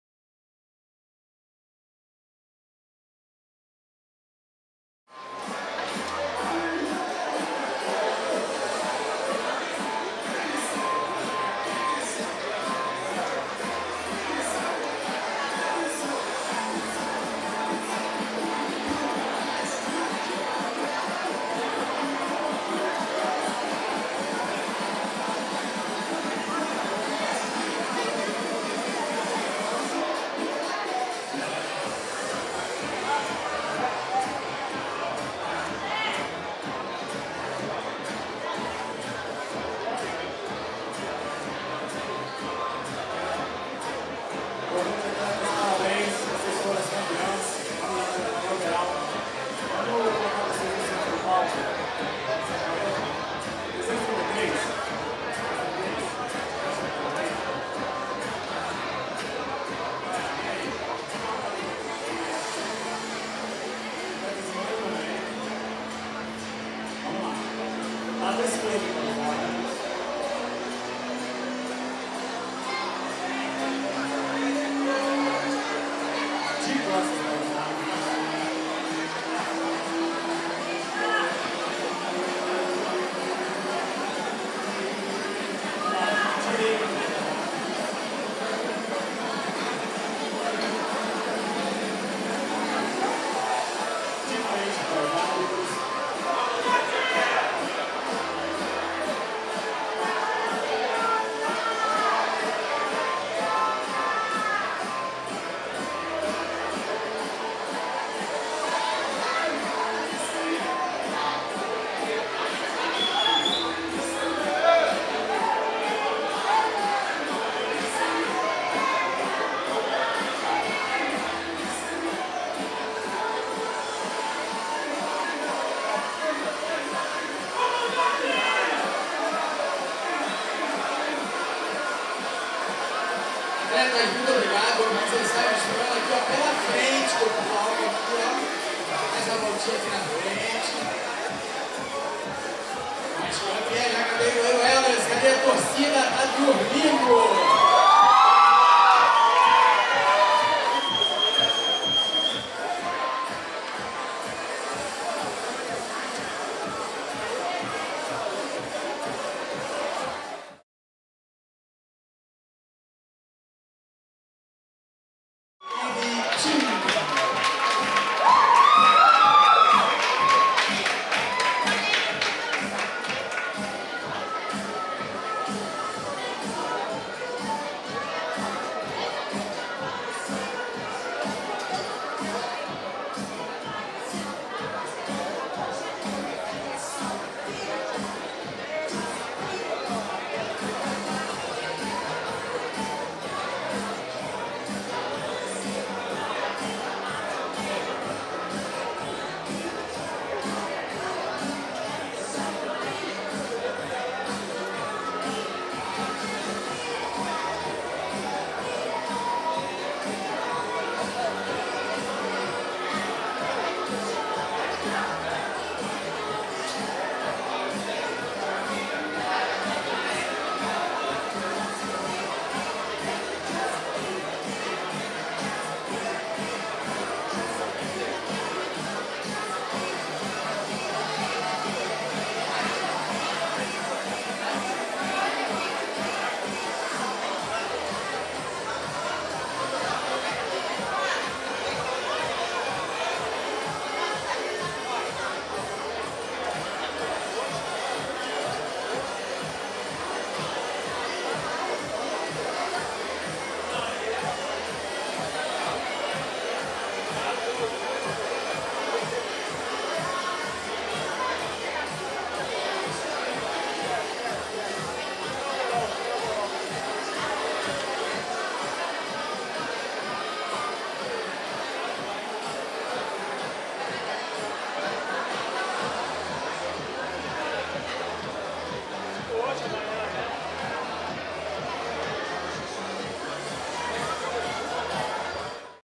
Mr Zona Oeste 2022 Overall Wellness Sênior
Transmissão ao Vivo do Campeonato Mr. Rio 2022 BRAFF, realizado nos dias 9 e 10 de julho de 2022, no Tijuca Tênis Clube na cidade do Rio de Janeiro.

Transmissão ao Vivo: Znit Produtora - @znitprodutora
Cobertura Fotográfica: Znit Produtora - @znit.ff

e saiba mais sobre as próximas transmissões Ao Vivo!